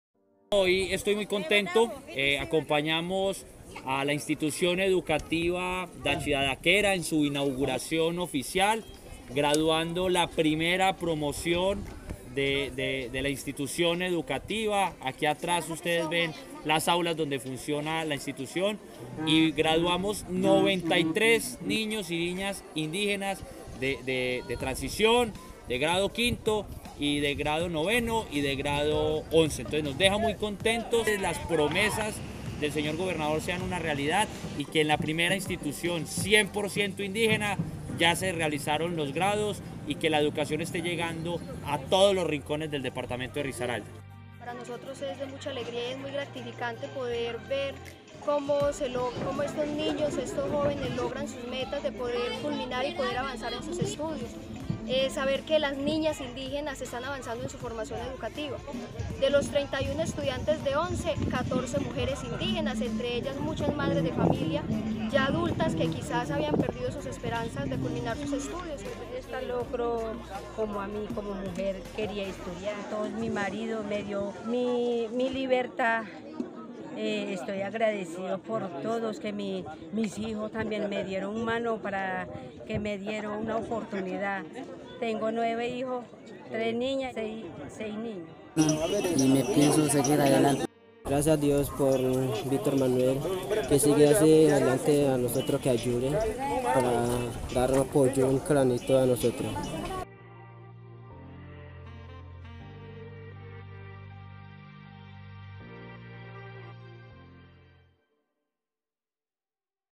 Grados en Kemberdé
Primera promoción en el Centro Educativo Dachi Dada Kera.